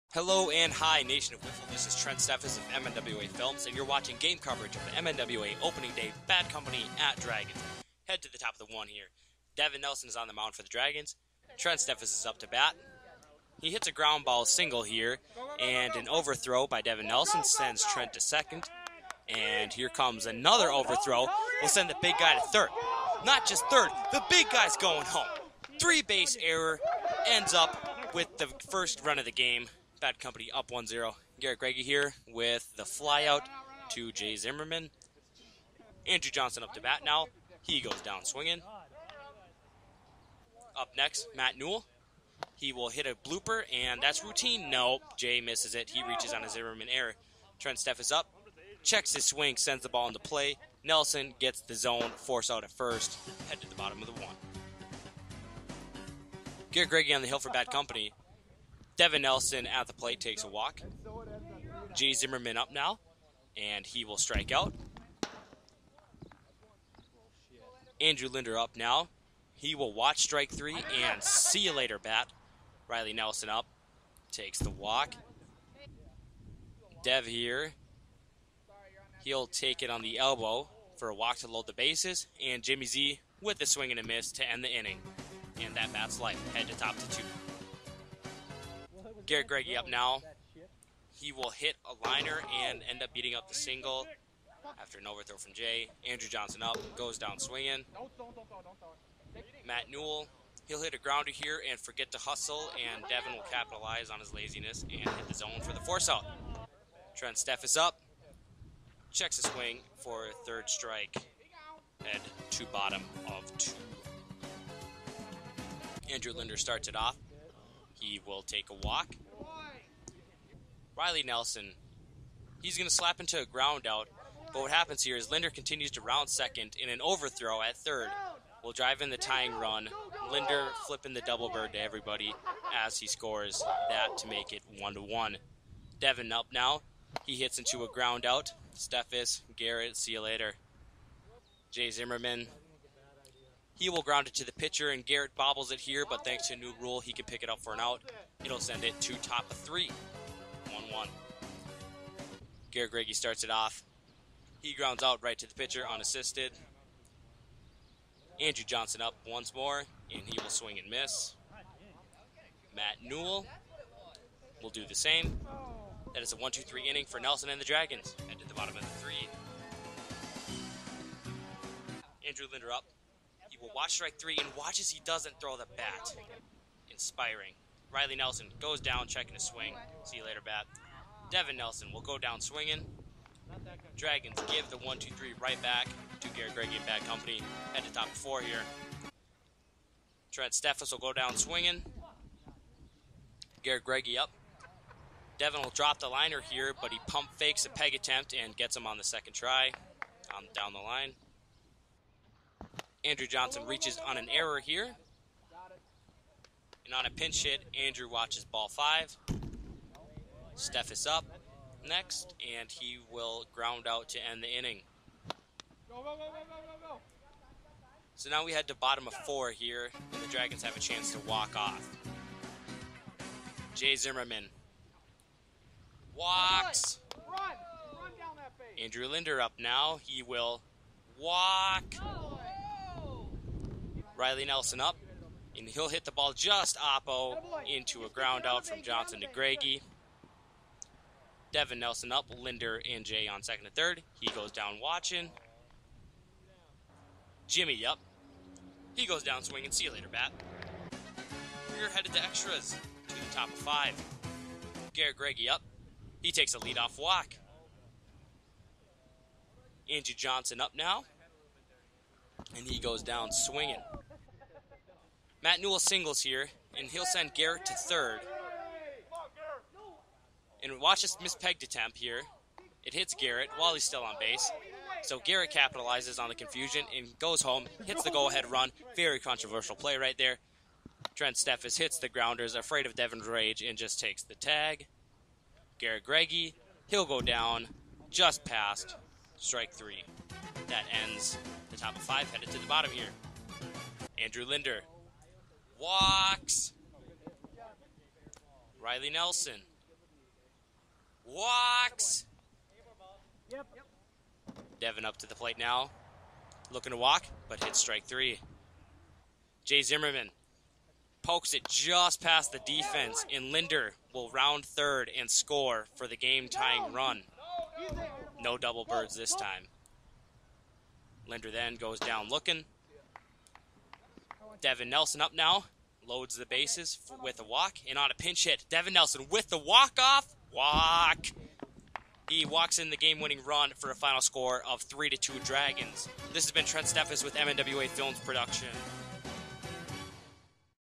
Opening Day: Bad Company @ Dragons
2015 MNWA Opening Day Coverage as the Bad Company (1-1) takes on the Dragons (0-2)